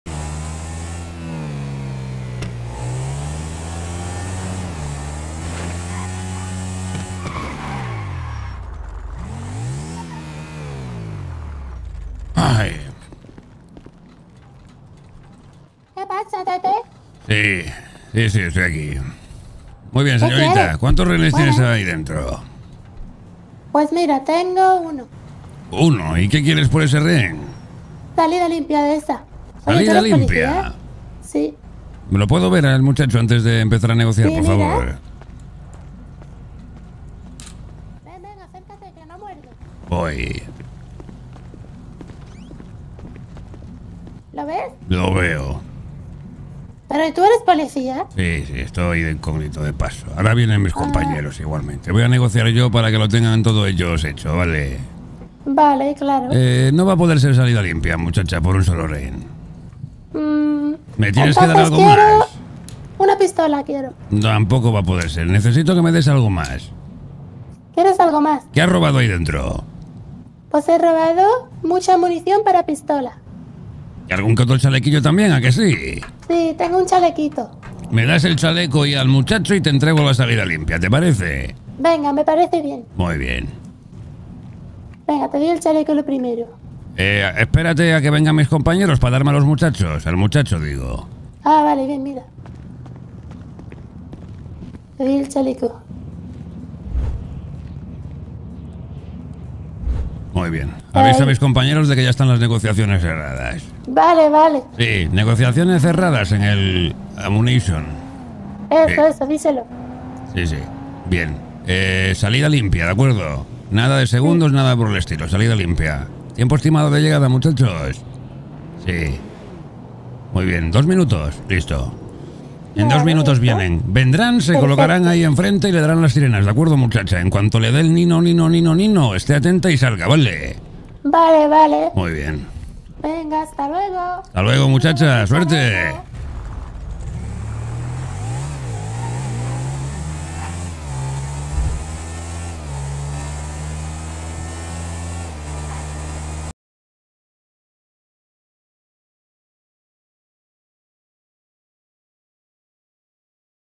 EL ABUELO SE HACE PASAR POR POLICÍA Y LE SACA UN CHALECO 😂
💜CANALES💜
desst3
deqiuv
imantado
axozer